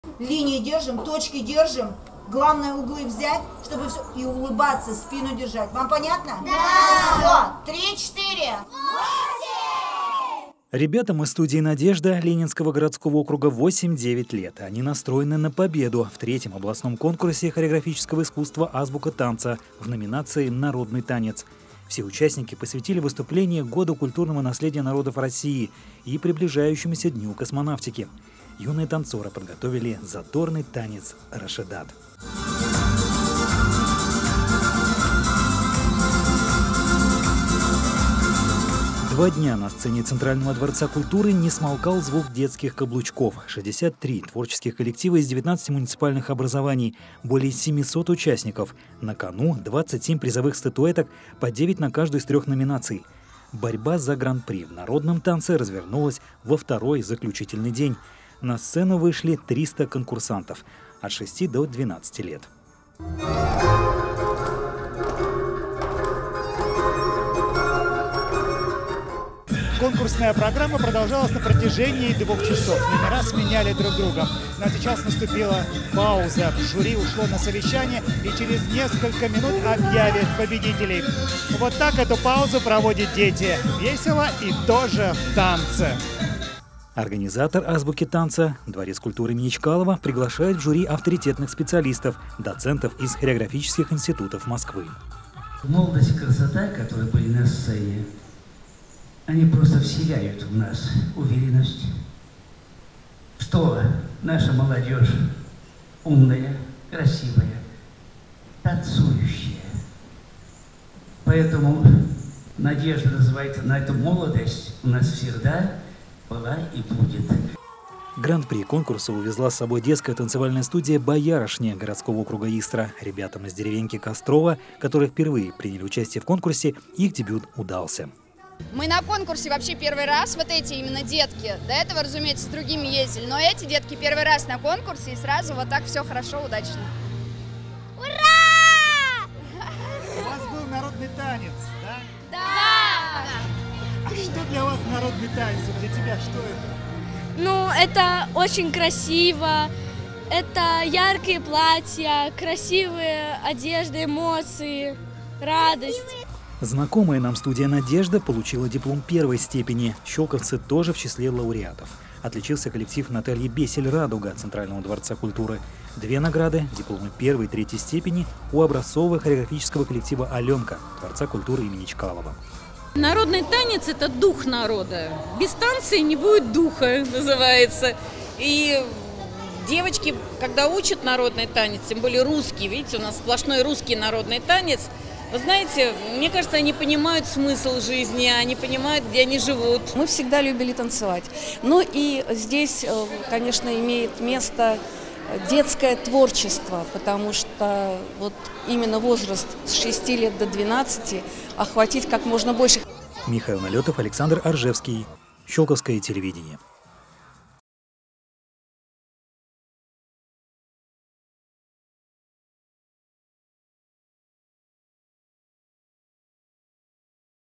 В Щёлково состоялся областной детский конкурс хореографического искусства «Азбука танца»
Зажигая звезды. Центральный дворец культуры принял участников Третьего областного детского конкурса хореографического искусства «Азбука танца». Кто из танцоров стал его дипломантом, а кому достался гран-при, смотрите в репортаже.